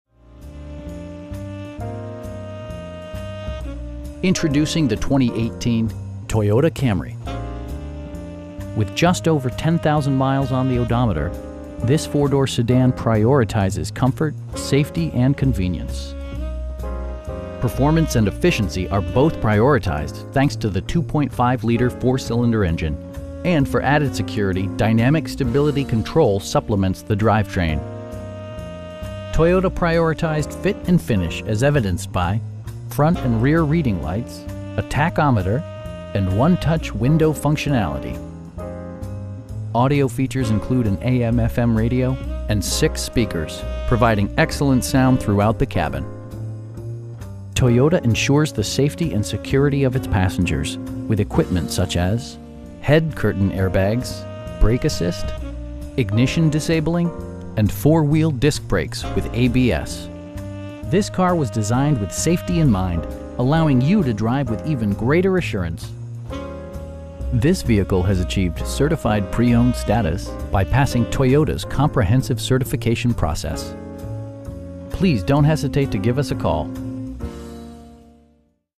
2018 Toyota Camry SE in Vienna, VA 22182
Introducing the 2018 Toyota Camry. With just over 10,000 miles on the odometer, this 4 door sedan prioritizes comfort, safety and convenience. Performance and efficiency are both prioritized thanks to the 2.5 liter 4 cylinder engine, and for added security, dynamic Stability Control supplements the drivetrain. Toyota prioritized fit and finish as evidenced by: front and rear reading lights, a tachometer, variably intermittent wipers, a trip computer, an outside temperature display, telescoping steering wheel, and 1-touch window functionality. Audio features include an AM/FM radio, and 6 speakers, providing excellent sound throughout the cabin. Toyota ensures the safety and security of its passengers with equipment such as: dual front impact airbags with occupant sensing airbag, head curtain airbags, traction control, brake assist, ignition disabling, and 4 wheel disc brakes with ABS. This car was designed with safety in mind, allowing you to drive with even greater assurance. This vehicle has achieved Certified Pre-Owned status, by passing Toyota's comprehensive certification process. We pride ourselves in consistently exceeding our customer's expectations. Please don't hesitate to give us a call.